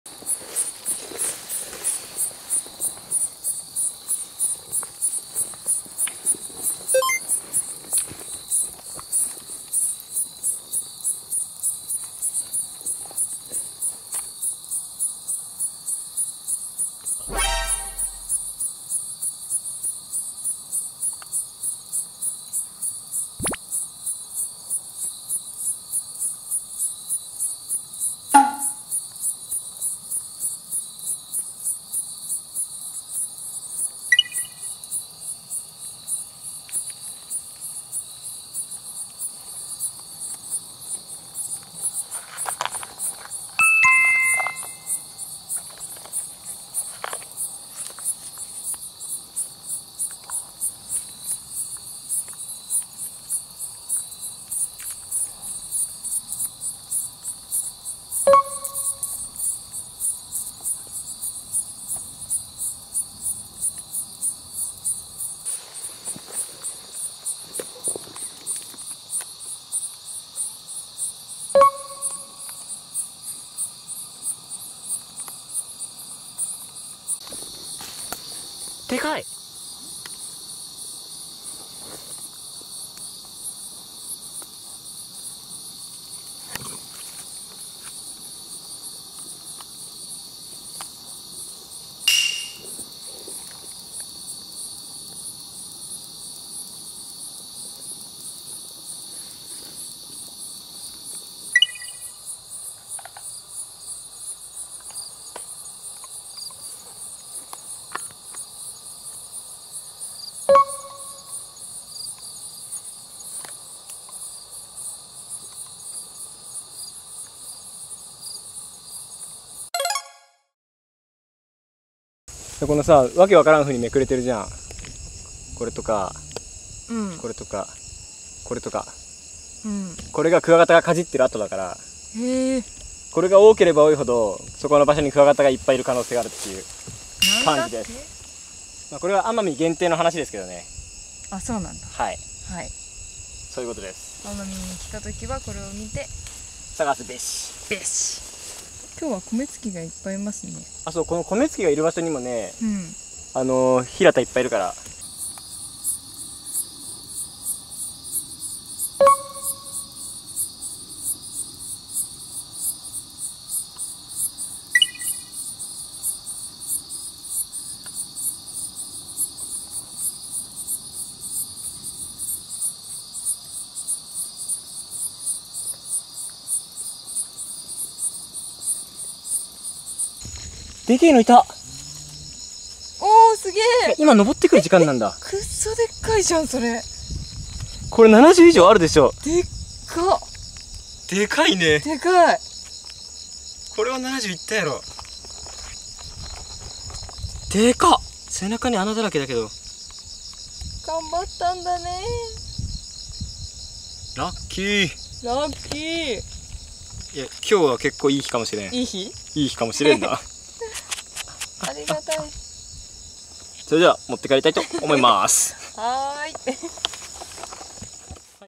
【クワガタ採集】死ぬまでに1度は見たいクワガタ採集（奄美大島）
今回は奄美大島での採集風景になります
アマミヒラタ
スジブトヒラタ
アマミコクワ
アマミノコギリ
サイカブト
が採集出来ました
応援イラスト・お便りはこちら！！
418-0111 静岡県富士宮市山宮２６６　TOP　GUN

いただいたお便り・イラストは動画内で挿し絵や紹介コーナーなどに使わせていただきたいと思います